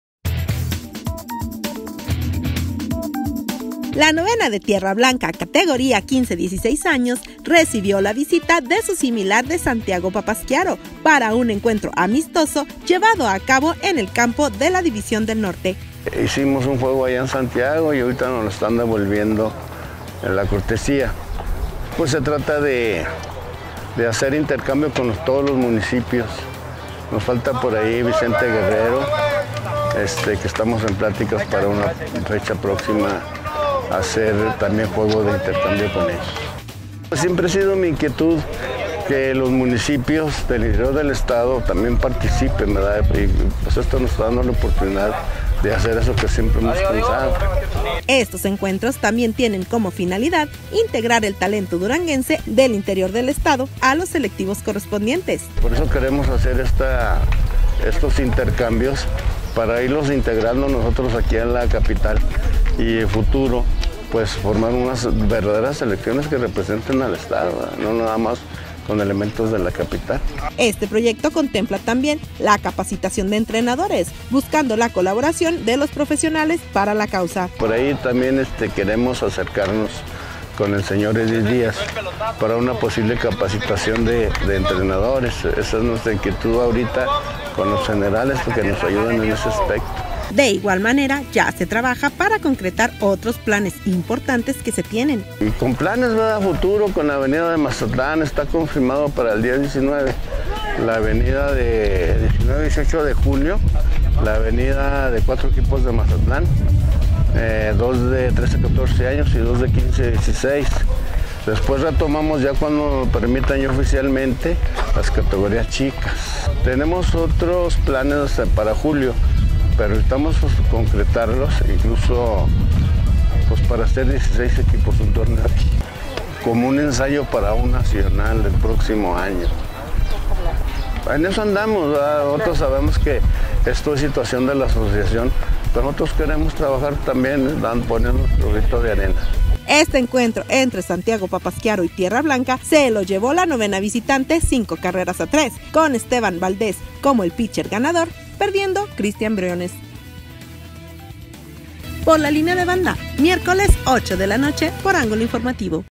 VISITA SANTIAGO PAPASQUIARO A TIERRA BLANCA EN BÉISBOL
Santiago Papasquiaro paga la visita a Tierra Blanca, dentro de la categoría 15-16 años y se lleva el encuentro 5 carreras a 3.